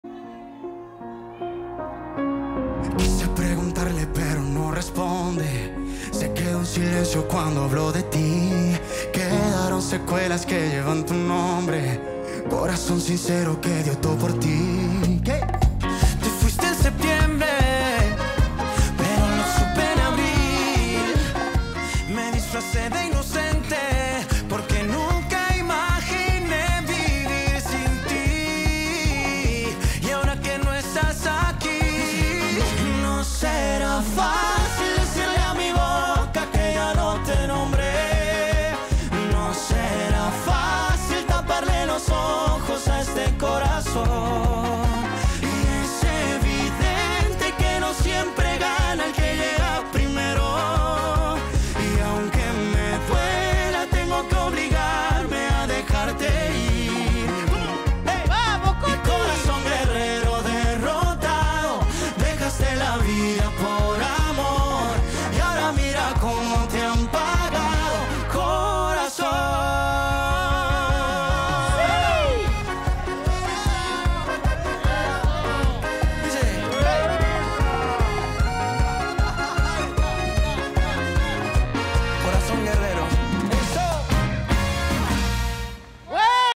Coty Romero y Jitsu Díaz abrieron la segunda ronda con un CUARTETAZO
💃 Coty Romero y Jitsu Díaz abrieron la segunda ronda del